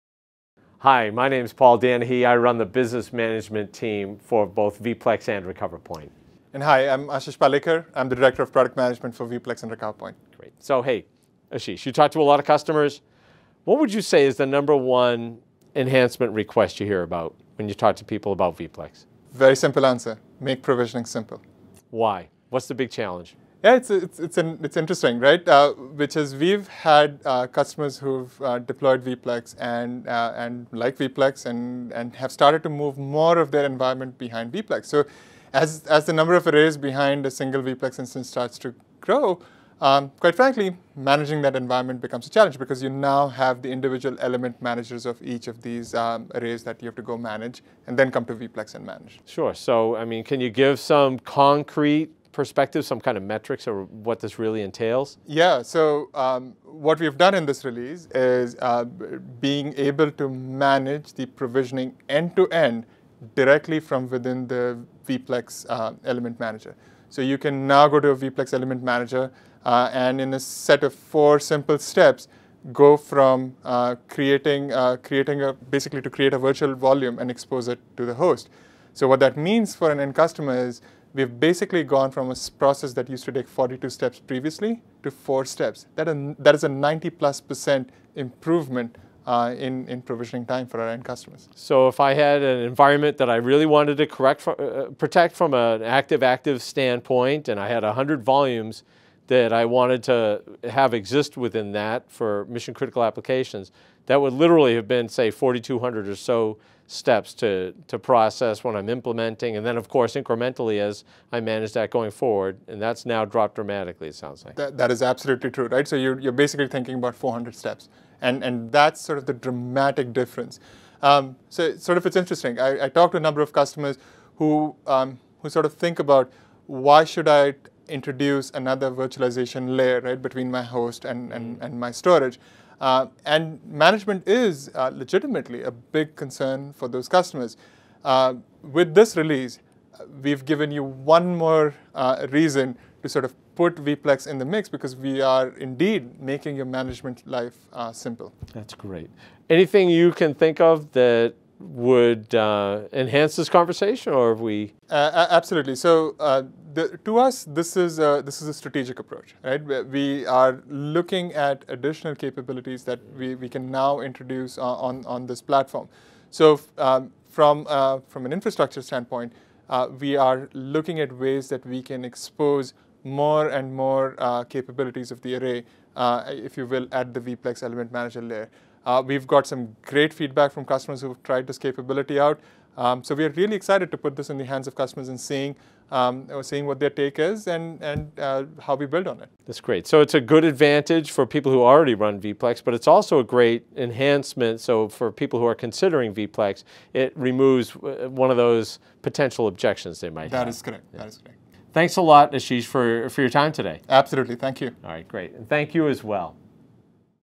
Simplified Storage Provisioning with EMC VPLEX
This video will discuss new end-to-end storage provisioning from within the VPLEX element manager. The number of steps to provision is now reduced to 4 simple wizard-based steps - a 90% reduction, making system management easier overall.